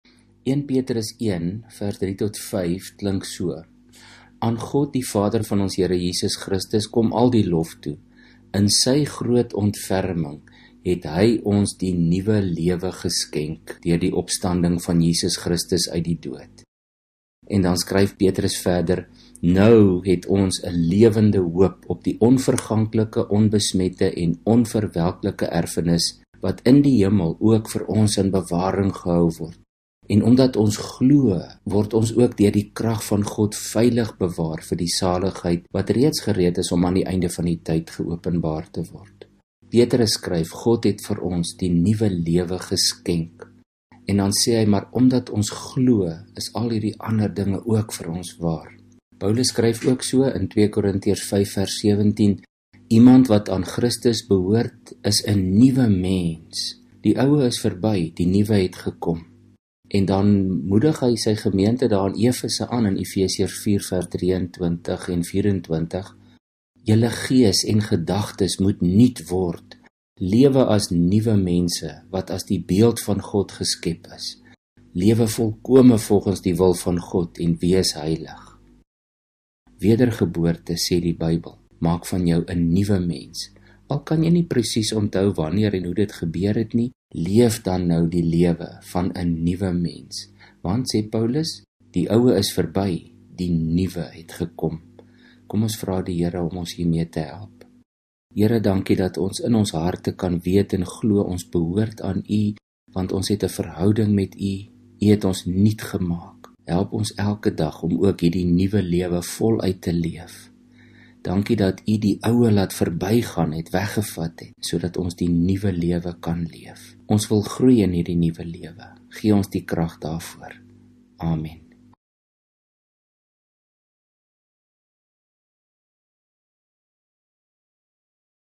Wedergebore beteken Nuwe Lewe! johannes vd merwe (ng kerk alkmaar) 16 junie
Wedergebore beteken Nuwe Lewe! johannes vd merwe (ng kerk alkmaar) 16 junie (1 petrus 1:3-5 en efes. 4:23-24) kort 2 min oordenking